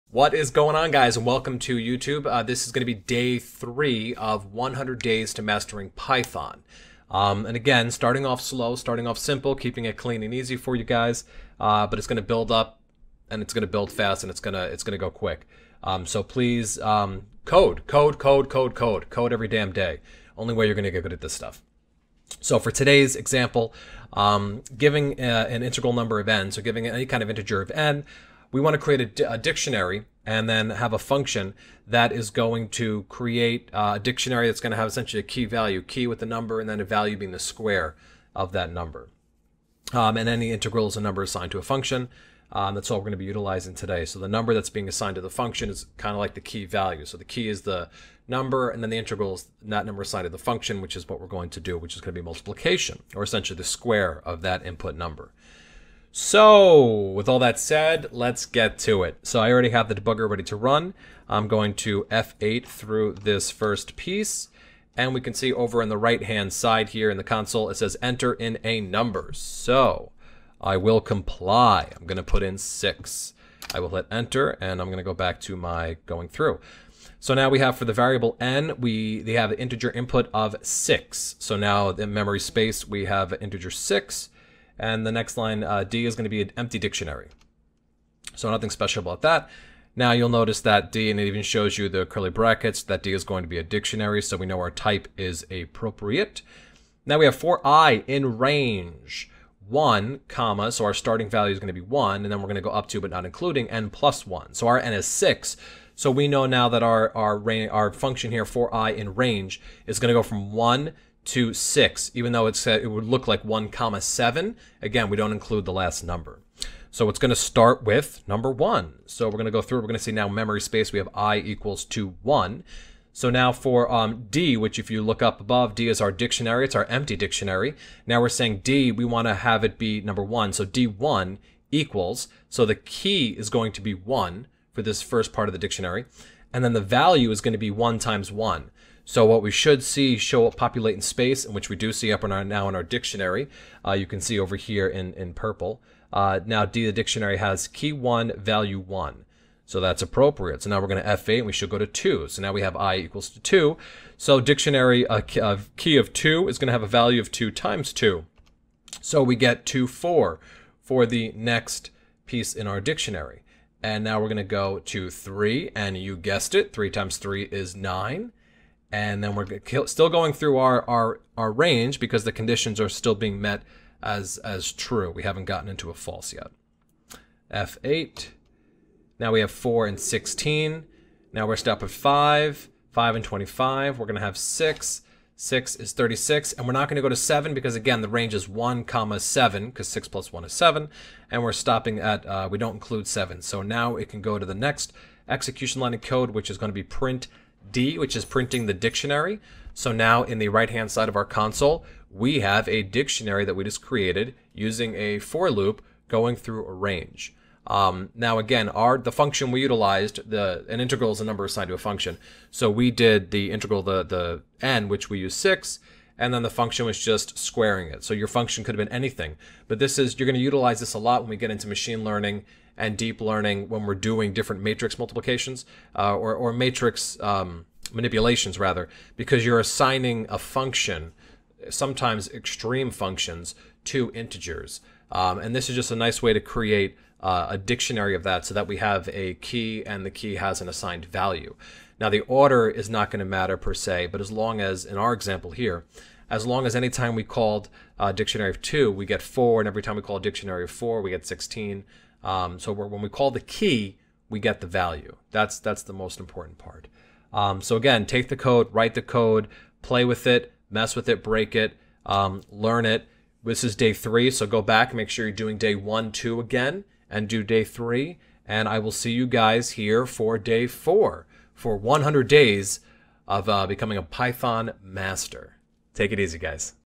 2017 - How To Learn Programming in 100 Days - Day 3
100 Days of python programming to become a master!  We are starting another playlist in programming where we will take a bar bone beginner starting from SCRATCH and getting them to python master in 100 days!